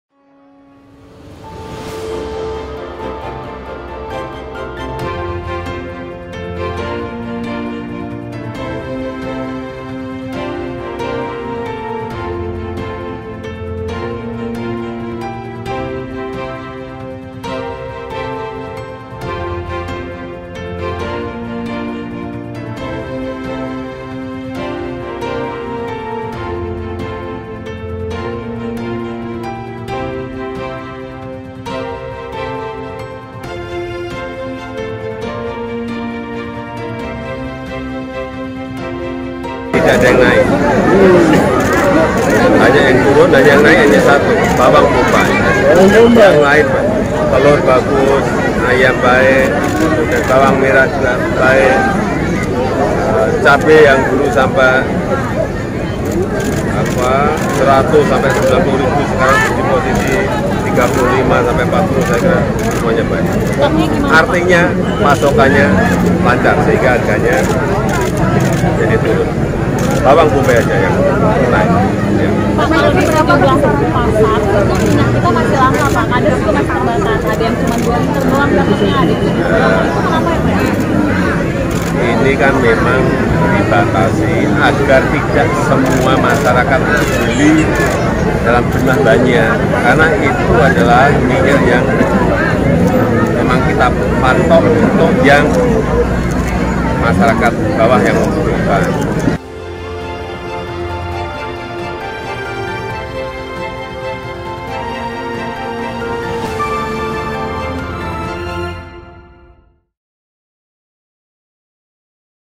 Jelang Lebaran, Jokowi Sebut Harga Bahan Pokok Alami Penurunan
Presiden Joko Widodo (Jokowi) menjelaskan harga bahan-bahan pokok di pasar mengalami penurunan. Hanya bawang bombai yang mengalami kenaikan harga.